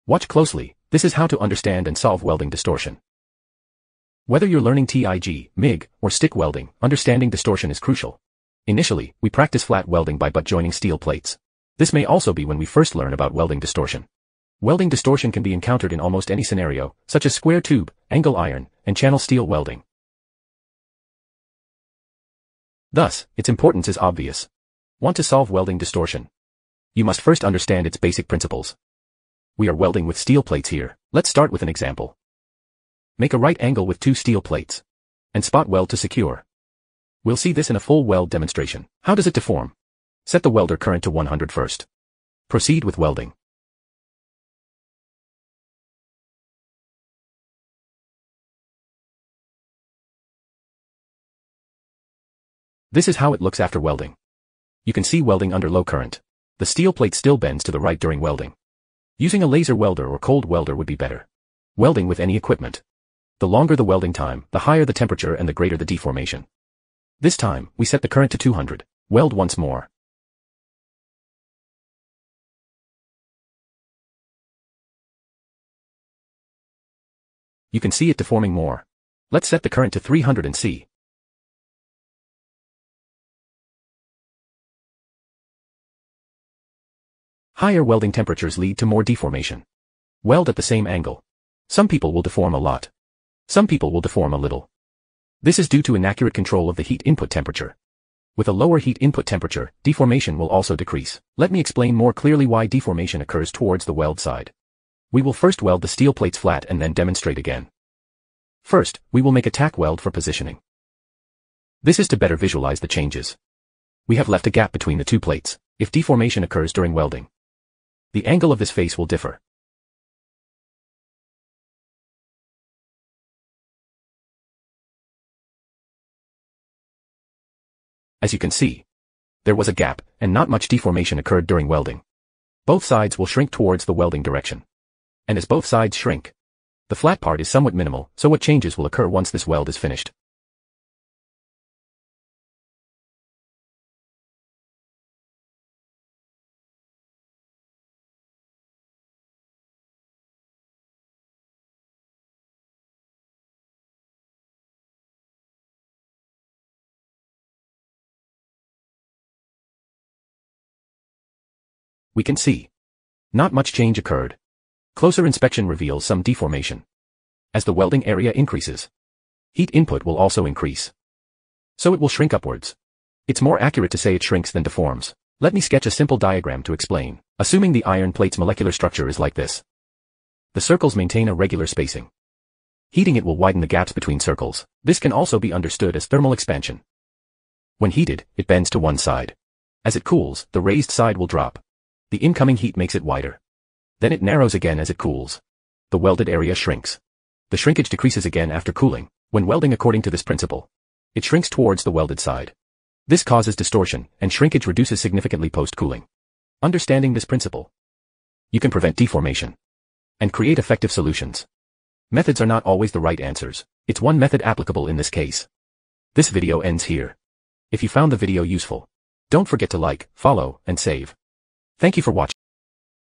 TIG Welding Teaching NO.33
When welding encounters deformation, how can we solve the problem and proceed with welding?